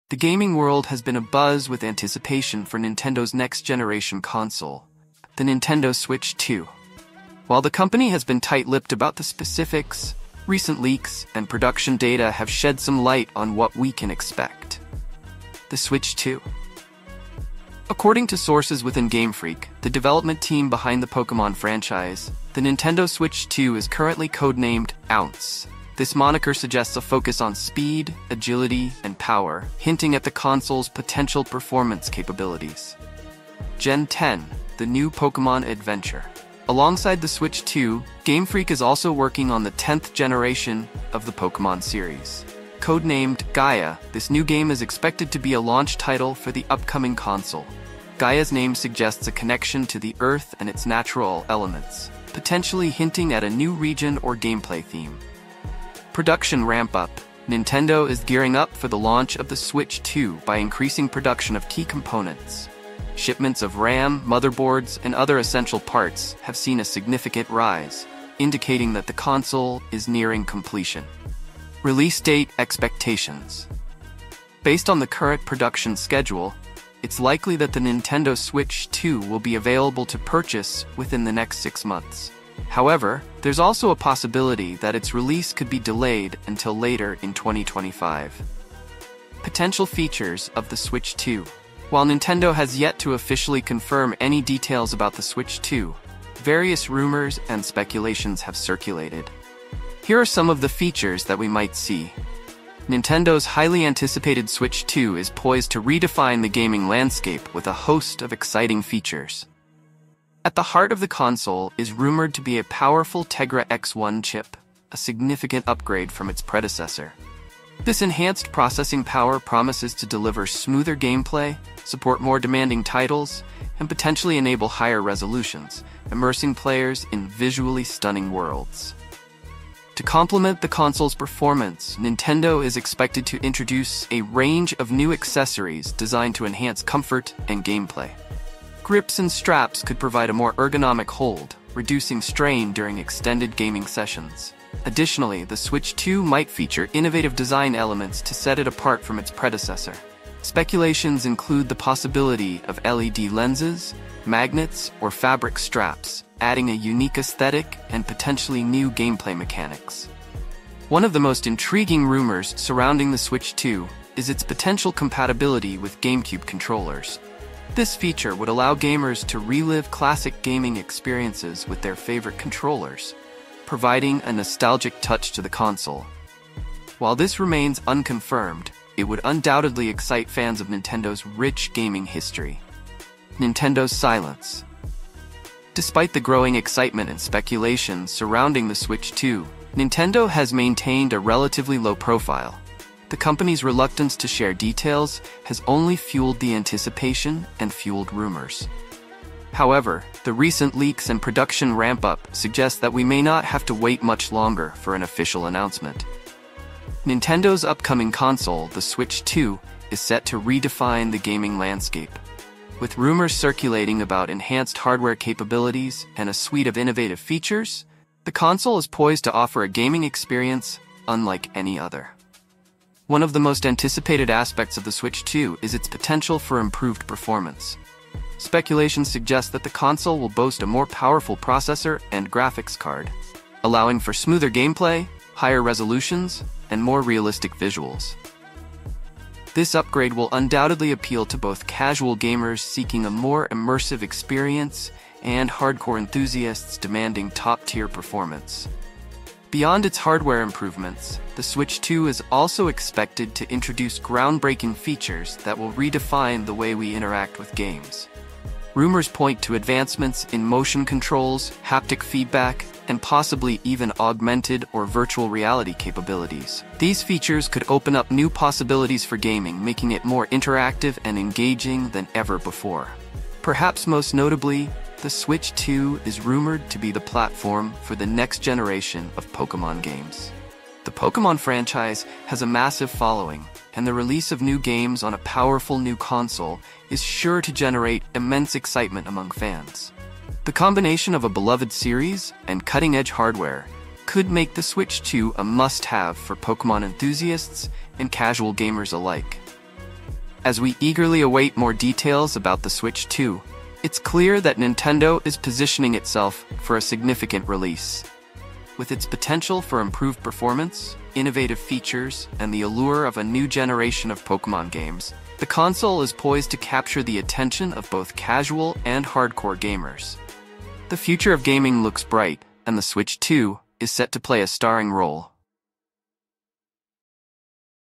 An interesting Nintendo Switch 2 update has just arrived!
Is the Nintendo Switch 2 Revolution finally coming? Rumors are swirling about the next-generation console from Nintendo, and we're diving into all the latest leaks and speculation. From improved hardware to new features, we're exploring what the Switch 2 could mean for gamers everywhere. Whether you're a fan of Mario, Zelda, or Pokémon, this video has everything you need to know about the potential Nintendo Switch 2 Revolution. Stay ahead of the curve and find out what's in store for the future of Nintendo gaming.